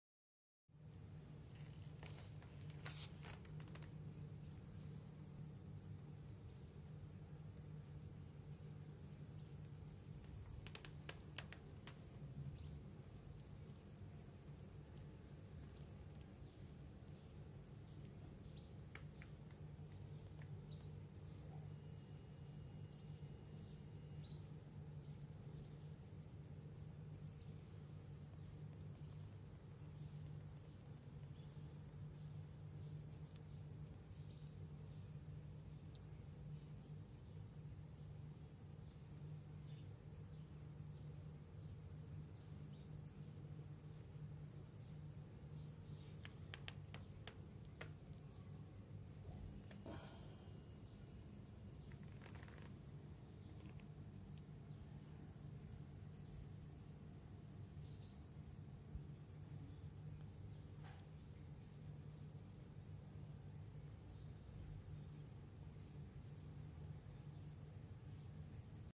Here's what happens when a Gecko's tail breaks off. Freaky!
I accidentally knocked this  gecko off the window ledge when closing a window in my house.

The impact of the landing broke off its tail, poor guy!

Hopefully s/he'll be ok, though more importantly; ITS TAIL IS CLEARLY POSSESSED!!!